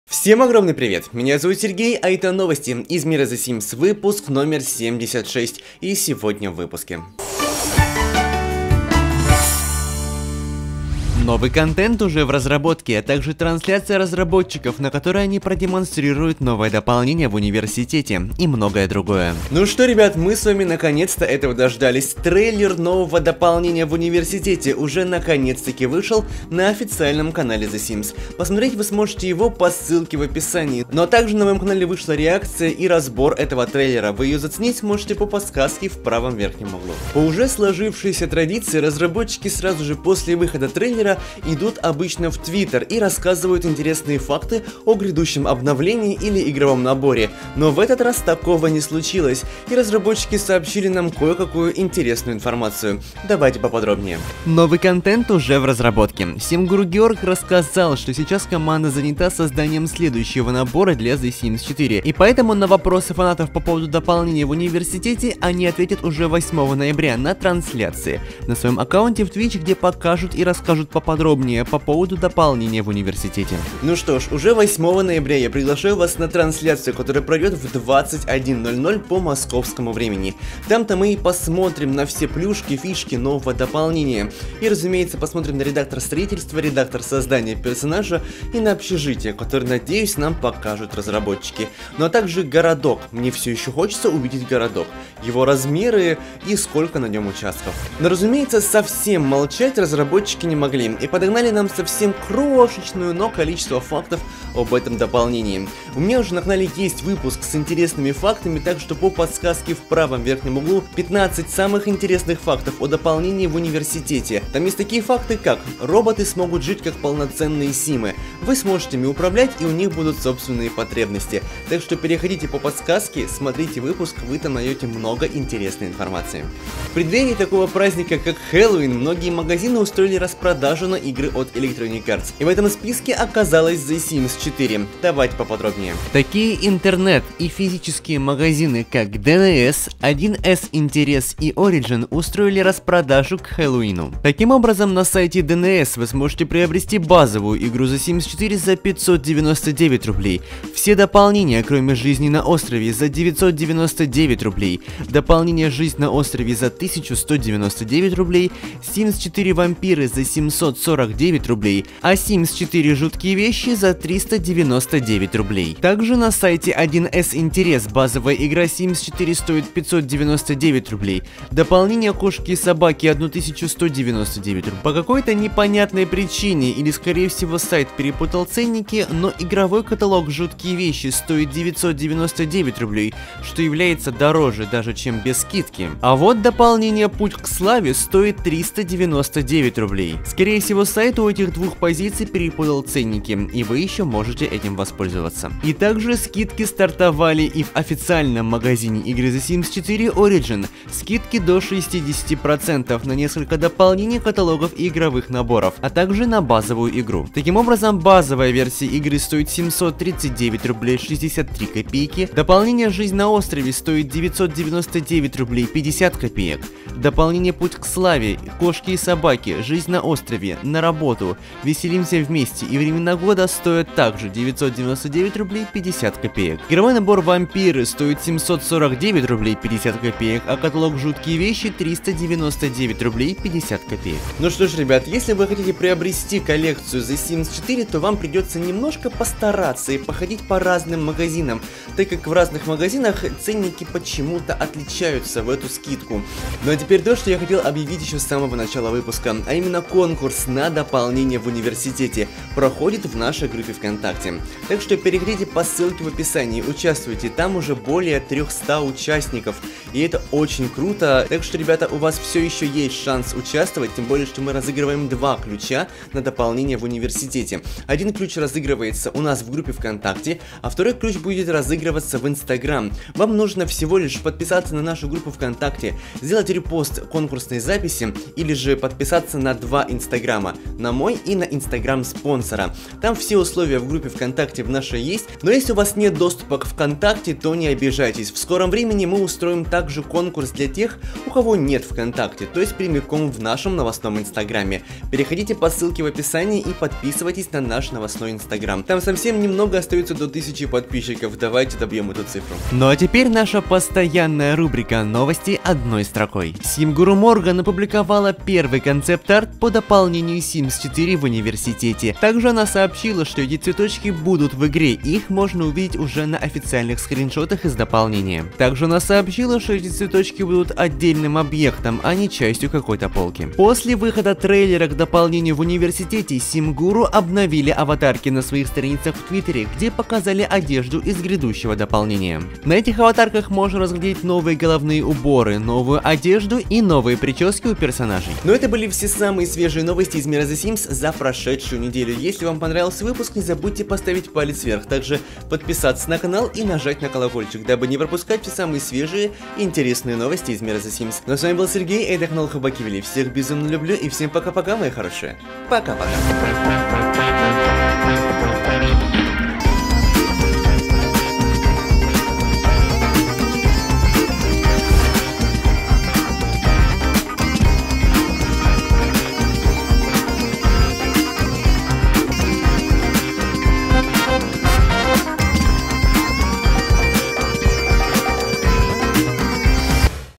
НОВОСТИ ИЗ МИРА THE SIMS : РАЗРАБОТКА НОВОГО НАБОРА | ТРАНСЛЯЦИЯ С РАЗРАБОТЧИКАМИ
Неделя подошла к концу, а это значит что пора подвести её итоги и рассказать о свежих новостях из мира The Sims!

Новости из мира The Sims выпуск №76
В этом выпуске мы поговорим о :
1)Разработка нового пакета
2)Трансляция разработчиков
и многое другое!
МОИ СОЦ.СЕТИ
● Steam : sergeishow
● Мой OriginID: sergyn20137